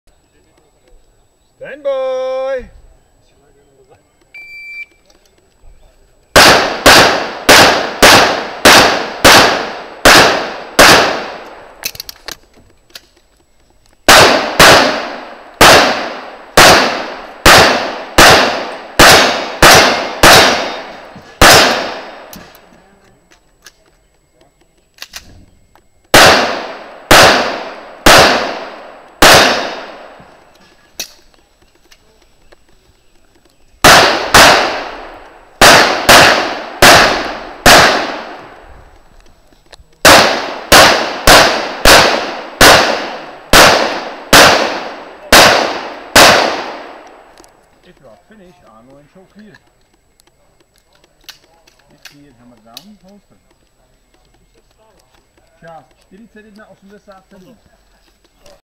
4. kolo ALSA PRO Moravský pohár 2014, Stage 5, Miluše Beránková, 44,05% na Jiri Behal
4. kolo ALSA PRO Moravský pohár 2014, Stage 5, Miluše Beránková, 44,05% na Jiri Behal